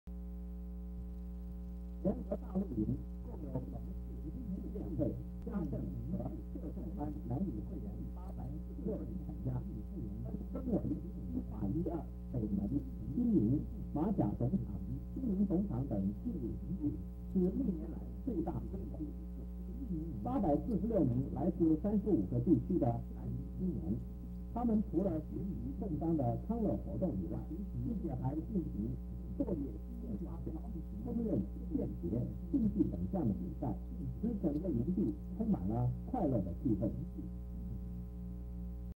台南縣農業推廣教育聯合大露營
(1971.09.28)聯合大露營將舉行兩天，北門鄉，新營鎮，學甲鎮，將軍鄉及後壁鄉等鄉鎮846名學員參加.除了有康樂活動以外，並舉行烹飪大賽，使整個營地充滿了快樂的氣氛.